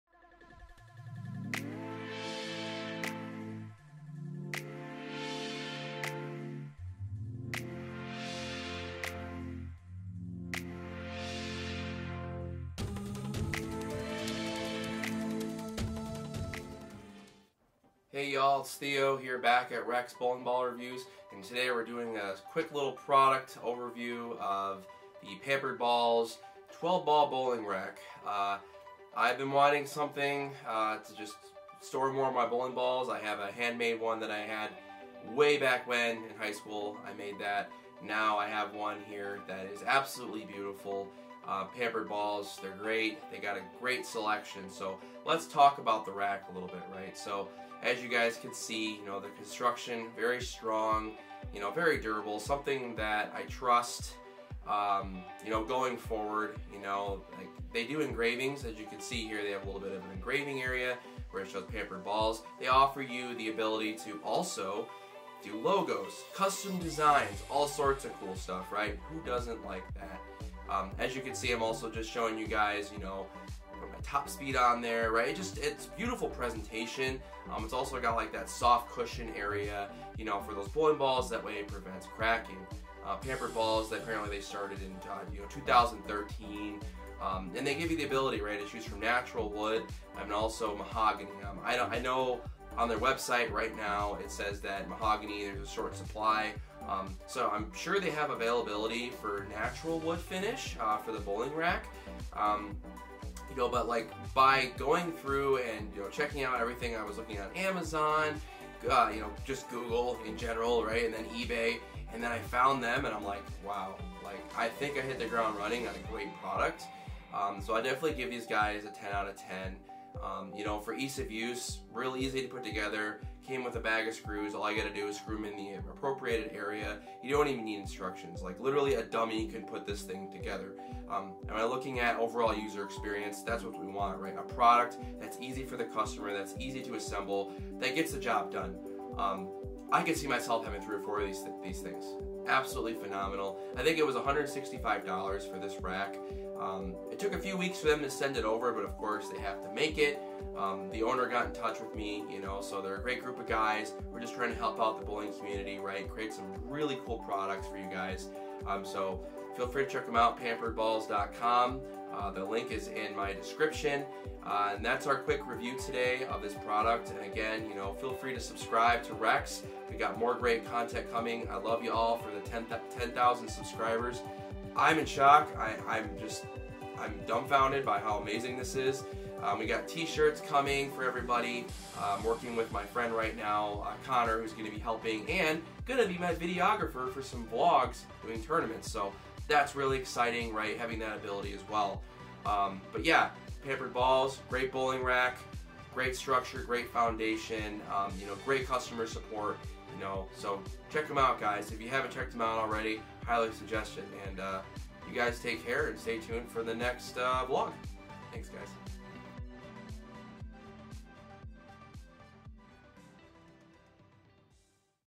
BEST BOWLING RACK EVER?!?! | REX Product & Accessories Review - Pampered Balls Bowling Rack 4K HD
Today, we review the Pampered Balls 12 Ball Bowling Rack! A great product that is reliable and easy to set up! You can choose from Natural or Mahogany wood, along with custom engravings and more! Theo snagged the natural wood version and does a bit of a usability/product walkthrough of the new bowling rack.

00:00 - Intro
00:60 - Natural Wood Finish
01:12 - Soft Texture to Prevent Cracking
01:59 - REX Product Review Score
02:28 - Price and Website